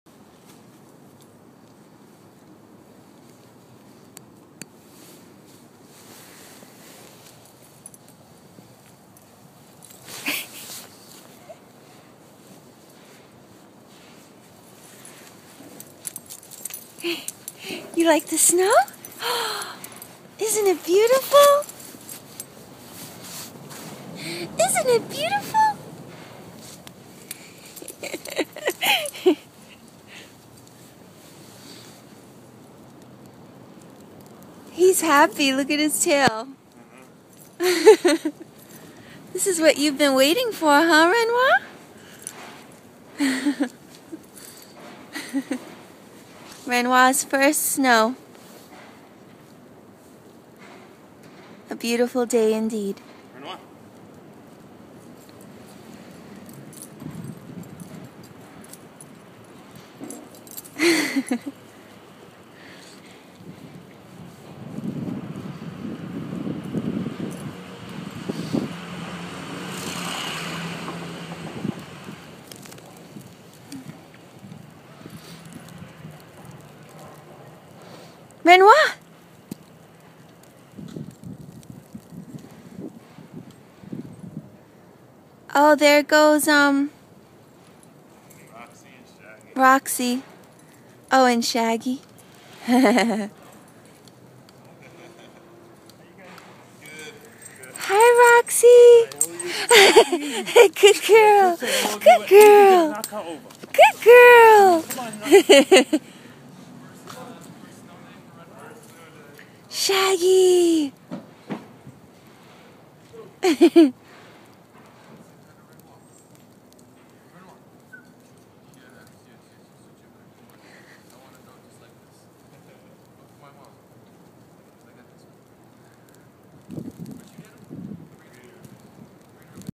Renoir's first snow
14 week old bouvier des Flandres playing in the snow!